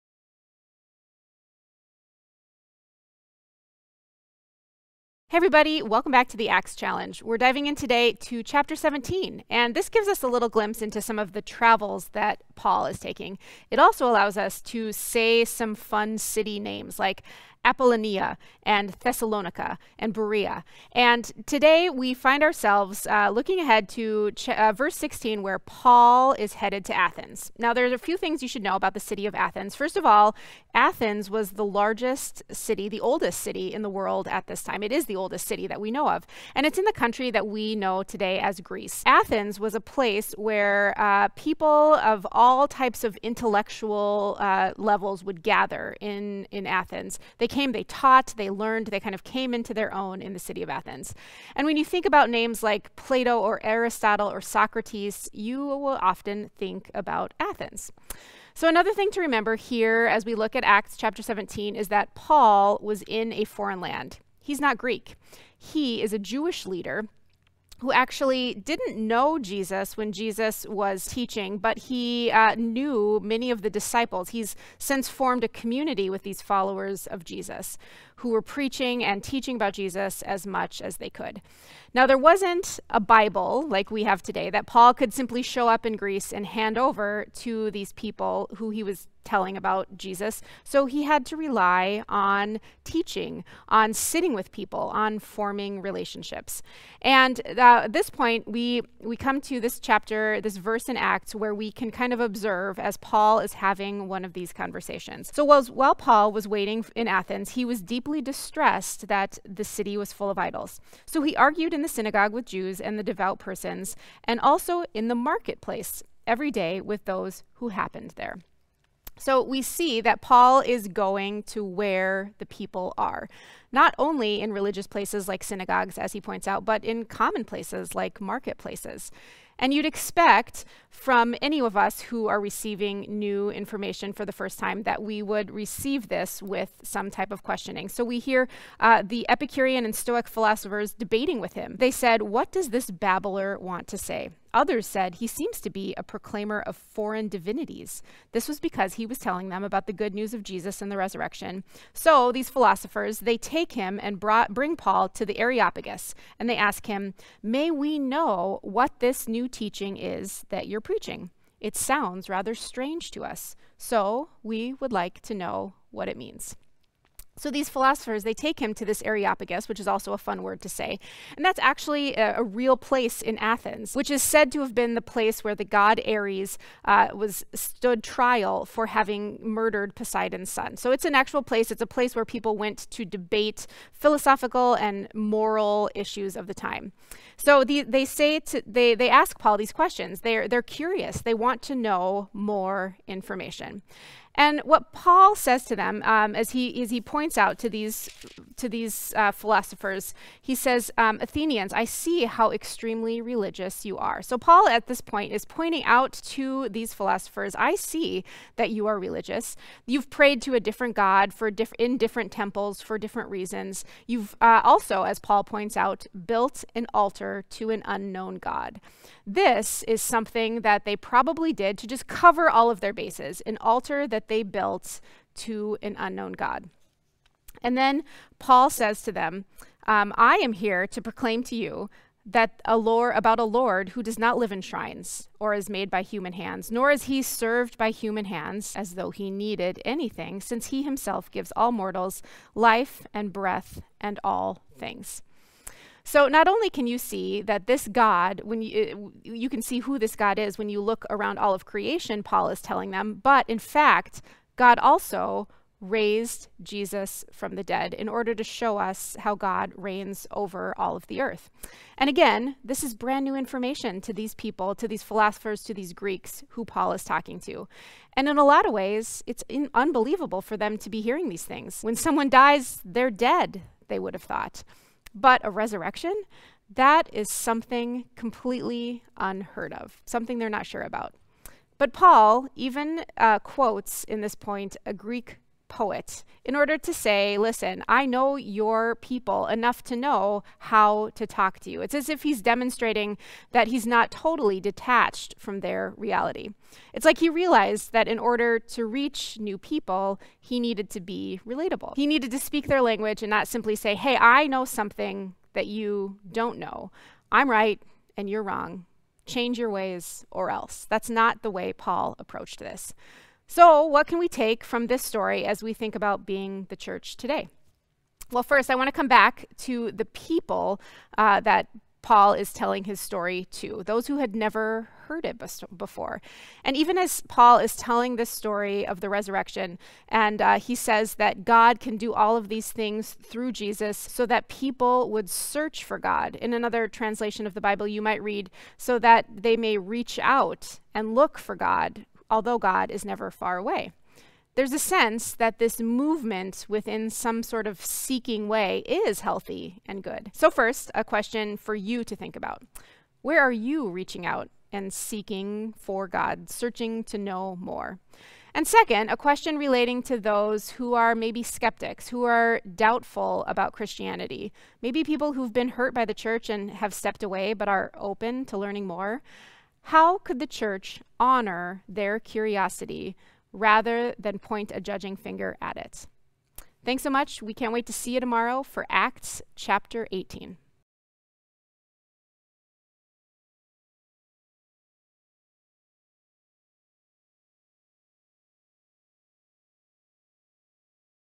Acts Chapter 17 | Honoring Curiosity About God
Paul ends up in foreign land in Acts 17 – the city of Athens in Greece. He meets people who are scholarly, religious people, but they’ve never heard about Jesus. In fact, they’ve built an altar to an unknown God, just to cover all their bases. And as Paul teaches them, he acknowledges that curiosity is a really healthy thing in a journey of faith.

1. Where are you searching to learn more about God?
2. How could the church honor curiosity, not judge?

SIGN-UP for text message reminders (M-F) as we journey through the book of Acts together!

Calvary Church is located in Alexandria, Minnesota.